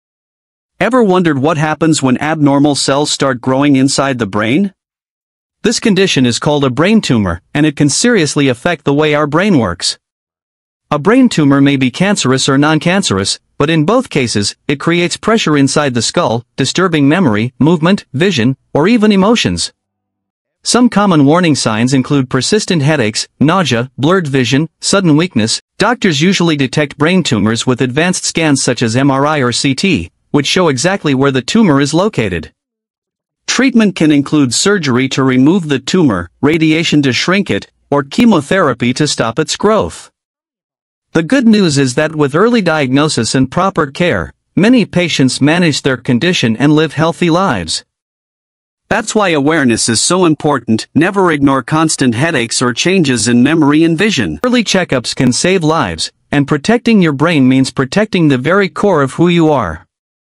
"Brain Tumor Explained | Causes, Symptoms & Treatment"🧠✨
In this short video, learn the basics about brain tumors — what they are, common symptoms, and possible treatments. If you or someone you know is experiencing symptoms, please consult a doctor immediately.